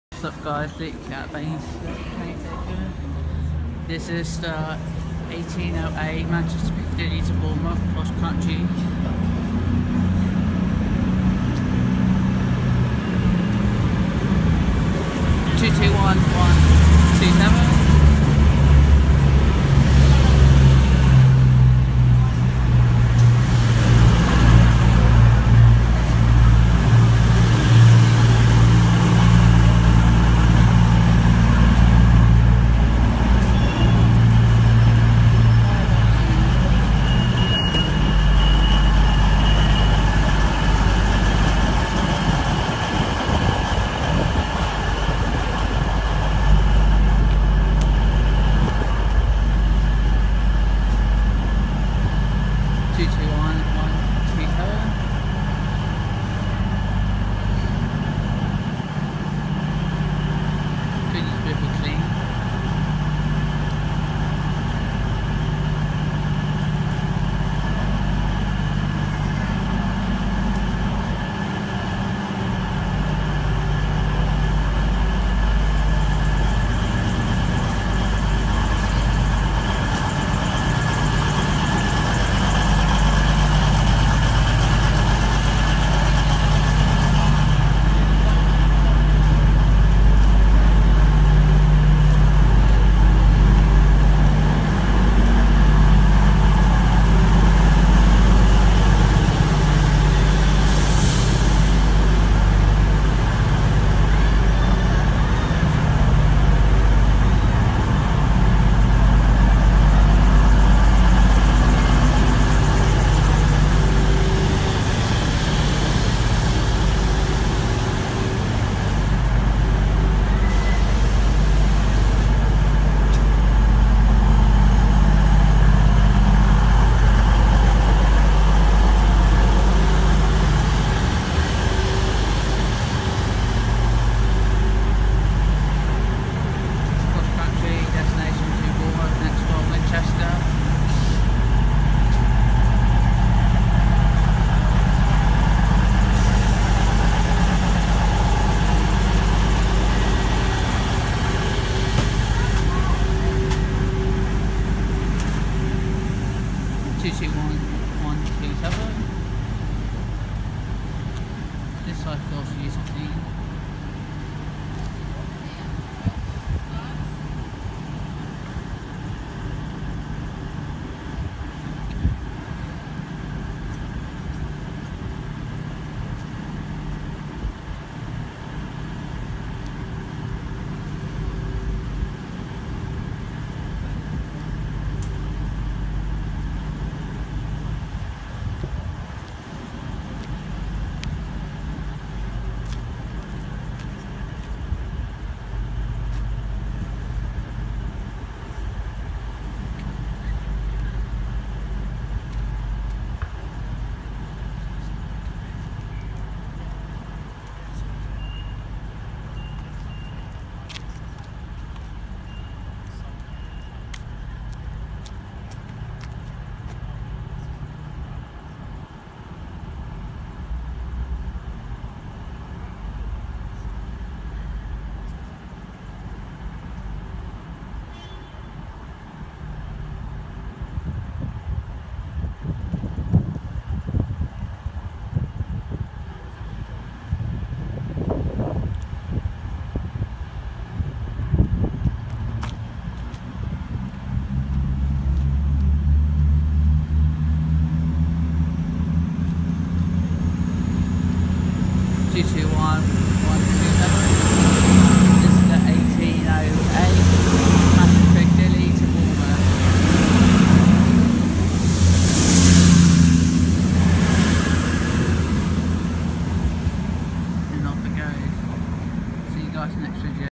XC Train at Basingstoke 18/4/17
Hey guys Luke here at Basingstoke train station doing a bit of train spotting hope you enjoy the video.